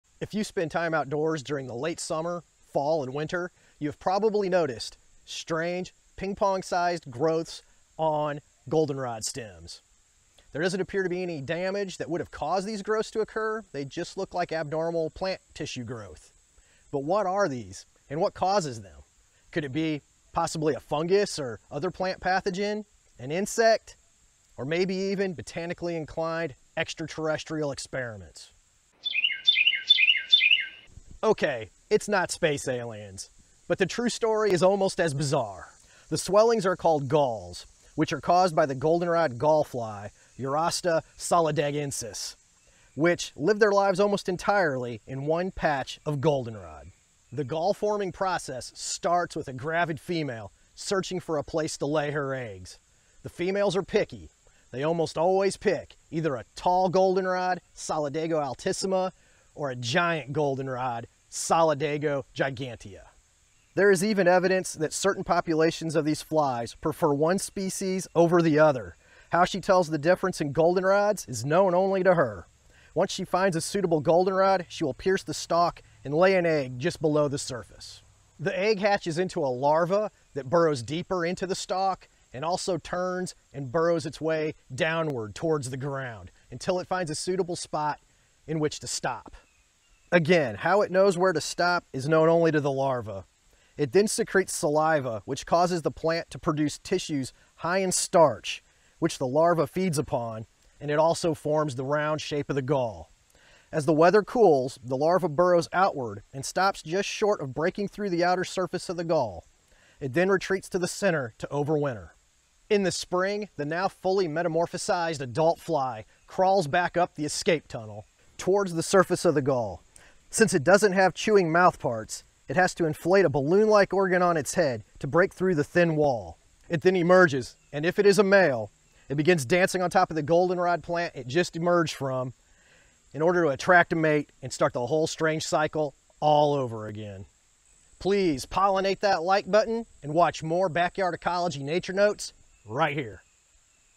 Goldenrod Galls - What, How, and Why
Ever wonder what those strange, round growths that appear on goldenrod in the late summer are? This video tells the what, how, and why of goldenrod galls and their cause, the goldenrod gall fly. A very cool true story about a super weird insect and a commonly found native plant.

Chapters:
0:00 Those Strange Round Growths on Goldenrod
0:31 Goldenrod Gall Fly Eurosta solidaginis
0:46 The Female Goldenrod Gall Fly Finds a Spot and Lays an Egg
1:17 Larval Stage of the Goldenrod Gall Fly
1:51 The Adult Goldenrod Gall Fly Emerges and the Cycle Continues